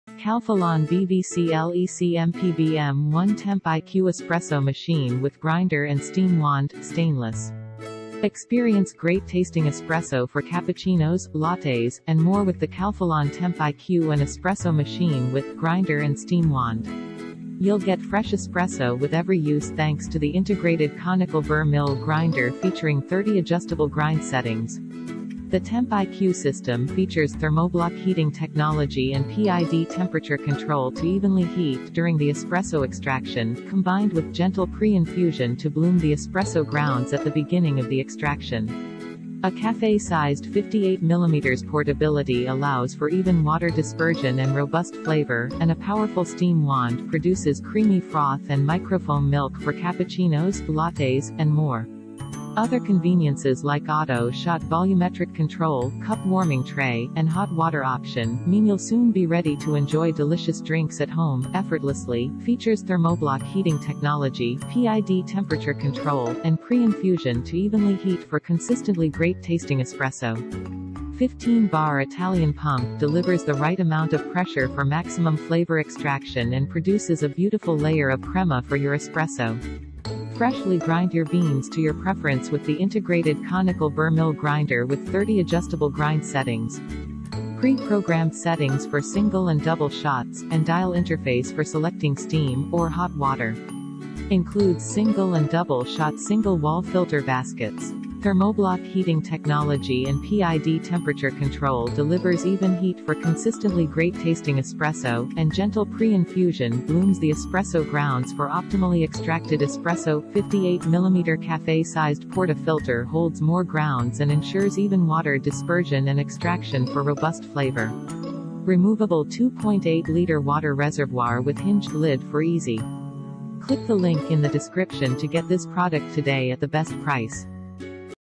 Calphalon BVCLECMPBM1 Temp iQ Espresso Machine with Grinder and Steam Wand, Stainless
Click here for the best price:

15-Bar Italian pump delivers the right amount of pressure for maximum flavor extraction and produces a beautiful layer of crema for your Espresso
Freshly grind your beans to your preference With the integrated conical burr mill Grinder with 30 adjustable grind settings
Pre-programmed settings for single and double shots, and dial interface for selecting steam, or hot water
Includes single- and double-shot single wall filter Baskets
Thermoblock heating technology and PID temperature control delivers even heat for consistently great-tasting Espresso, and gentle pre-infusion blooms The espresso grounds for optimally extracted Espresso
58millimeter café-sized porta filter holds more grounds and ensures even water dispersion and extraction for robust flavor
Removable 2.8 liter water reservoir with hinged lid for easy filling
Cup warming tray for espresso-ready cups and steam wand froths milk for creating cappuccinos, lattes, and other drinks, just like in coffee shops
Includes Stainless steel milk jug, tamper, cleaning disc, cleaning brush, and cleaning pin